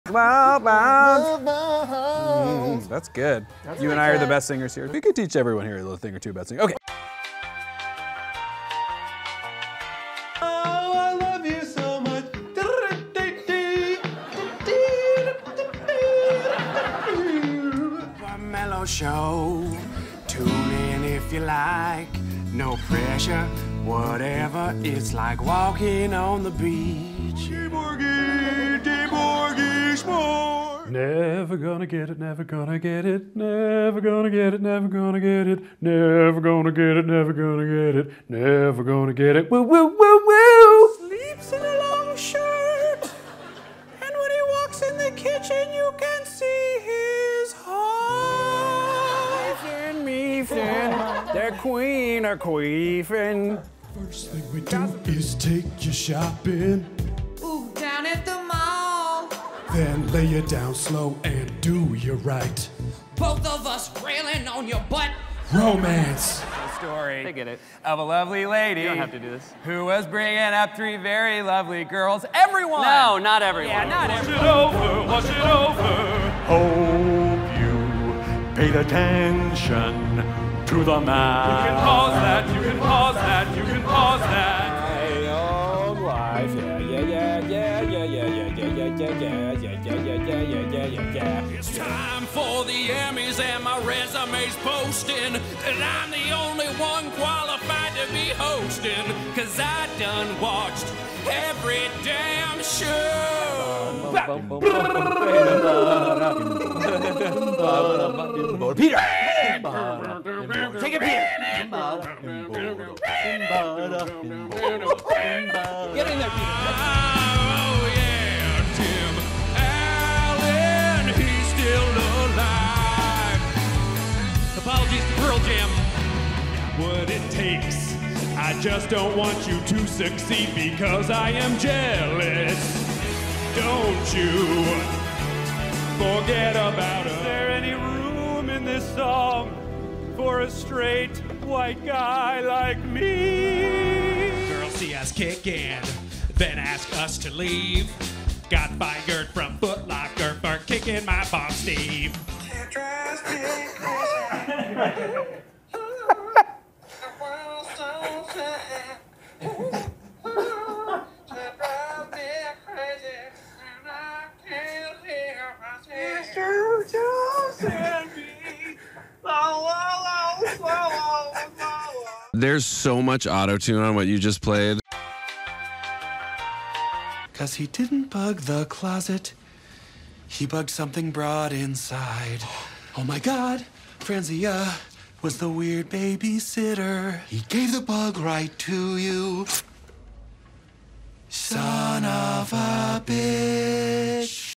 Andy Samberg's Singing Moments
Andy Sambeg the singer!

Some rare or the best moments of this talented and hilarious man singing.